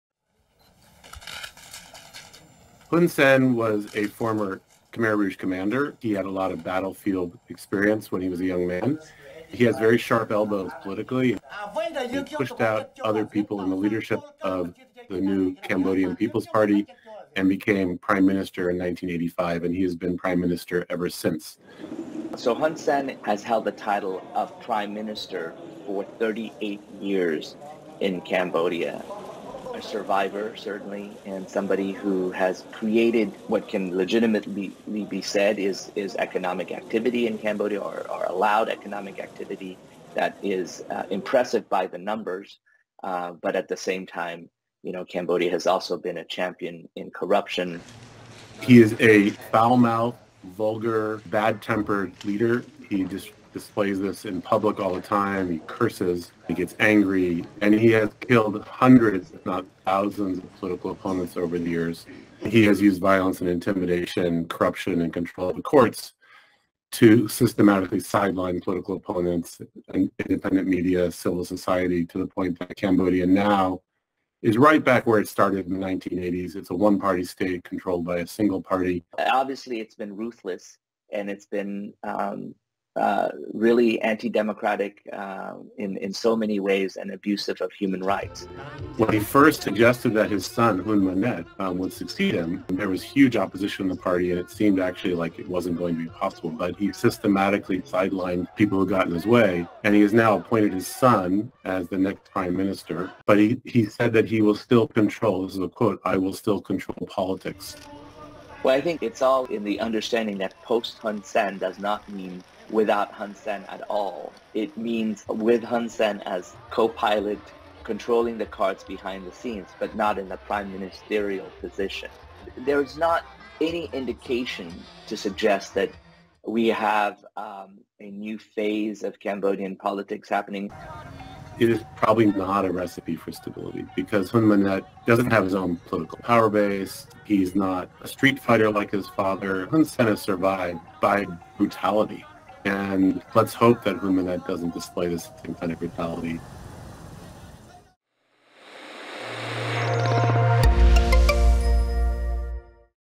“Survivor” Hun Sen wraps up 38 years as Cambodia’s leader Radio Free Asia RFA
Sophal Ear: “Survivor” Hun Sen wraps up 38 years as Cambodia’s leader   Radio Free Asia RFA